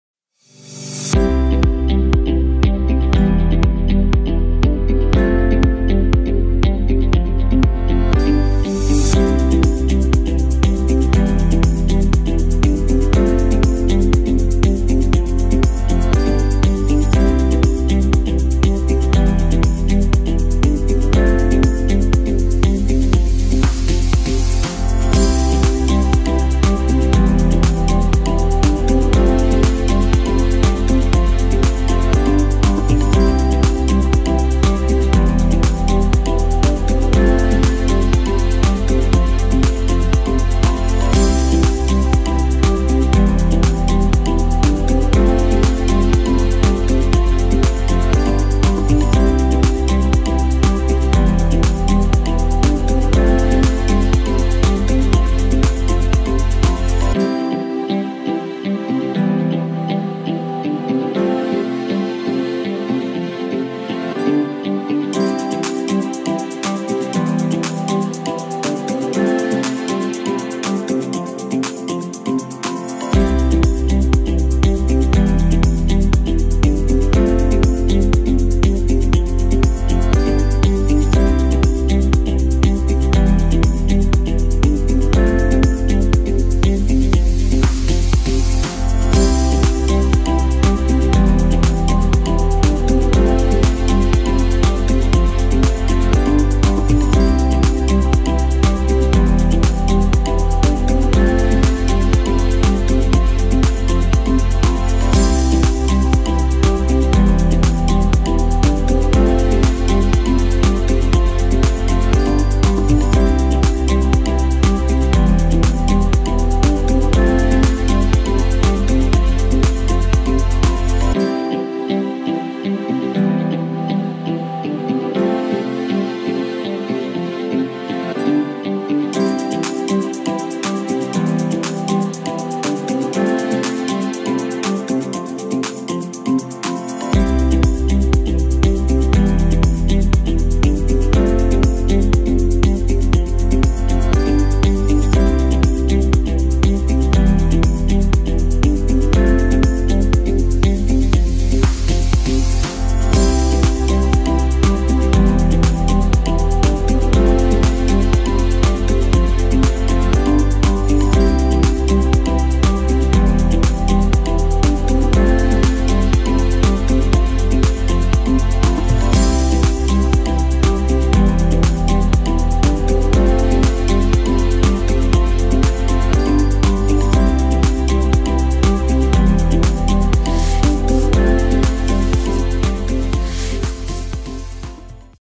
Recette aux fraises surgelées! Le cheesecake que je fais en toute saison
Ingrédients:
pour la base du gâteau:
jaune d’oeuf - 1 pièce
sucre vanillé - 10 g
sel - 3 g
beurre fondu - 175 ml
Au four à 180 ° C pendant 15 min

pour la composition aux fraises:
fraises surgelées - 500 g
eau - 50 ml
fécule - 30 g

pour la crème:
fromage à la crème - 500 g
crème liquide - 200 ml

Au four à 120 ° C pendant 60 min
Diamètre du moule 20 cm